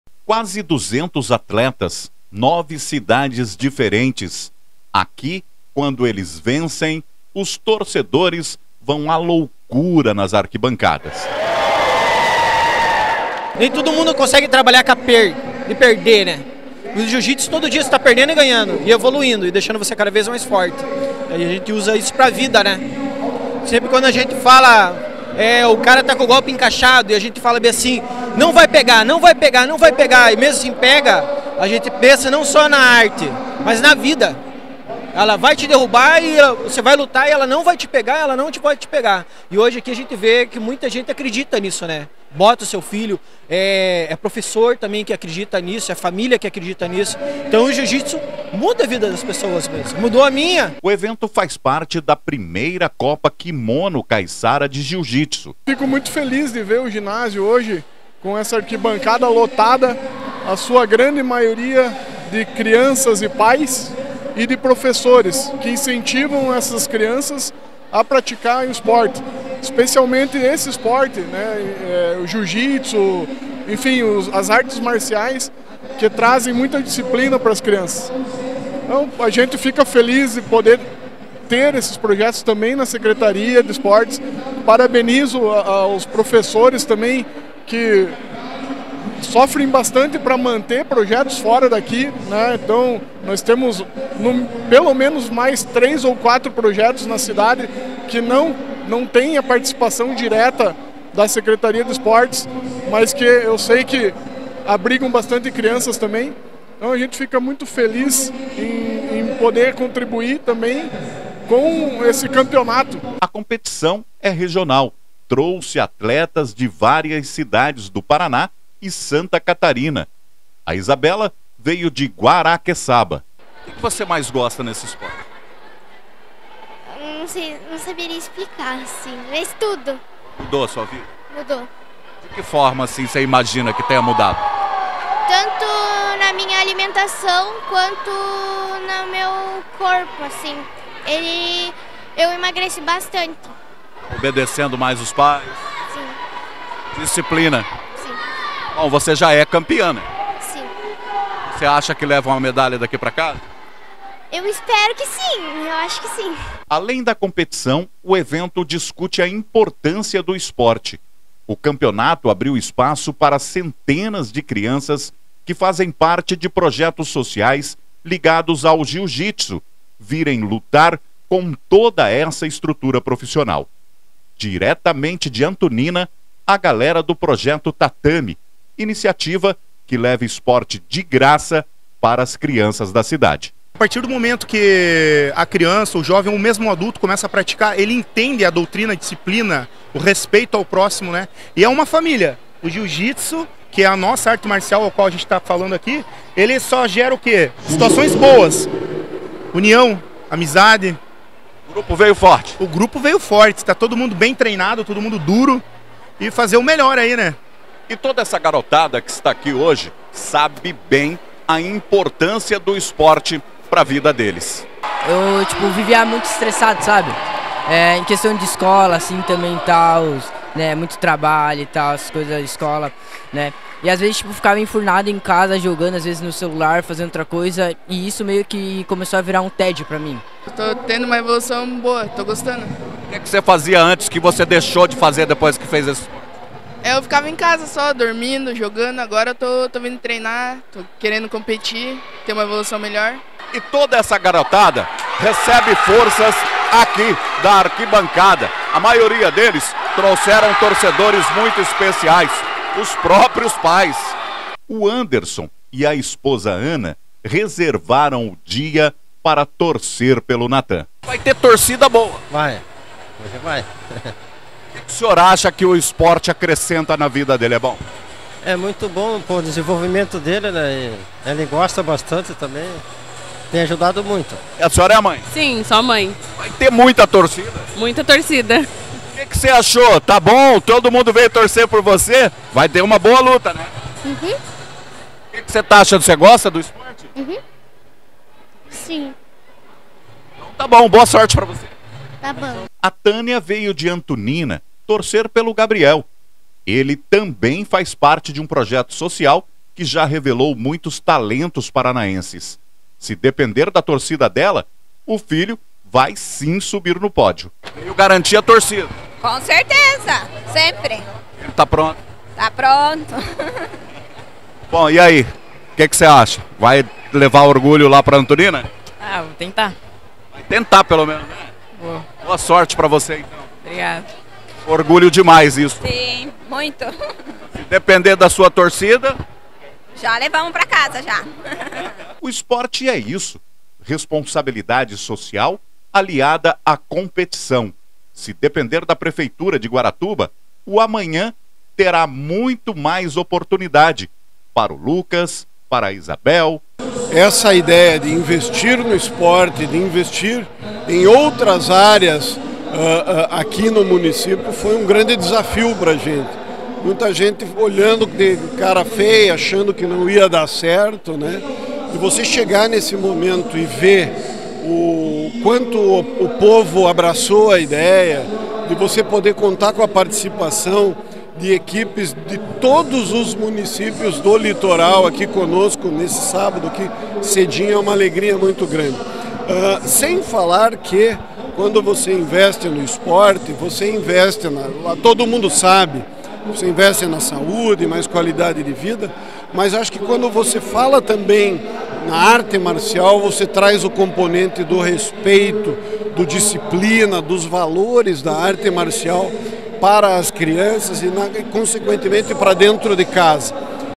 A Prefeitura de Guaratuba promoveu, a 1ª Copa Kimono Kaiçara de Jiu Jitsu 2023.
. A competição reuniu um total de 168 atletas vindos de 9 cidades do Paraná e de Santa Catarina. Organizada pela Secretaria Municipal do Esporte e do Lazer, o evento contou com as categorias Pré-mirim e Infanto-juvenil.